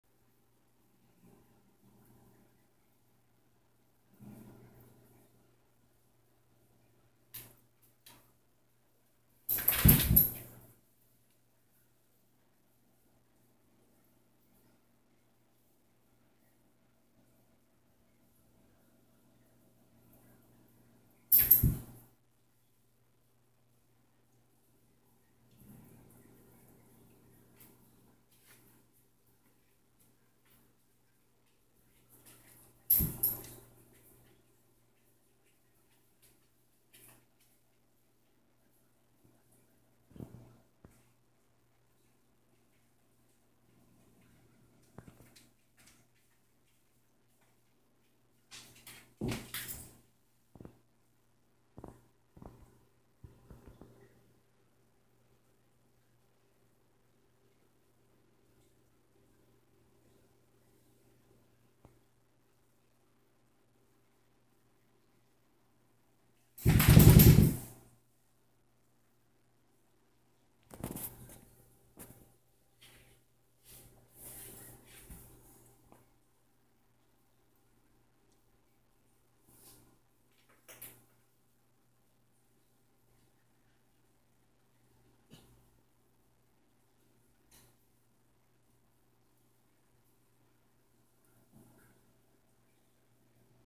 Our Golden Shark Catfish At Play
Our Golden Shark Catfish at play...we don't typically see him all that often, but after rearranging his tank...we got to see him dance!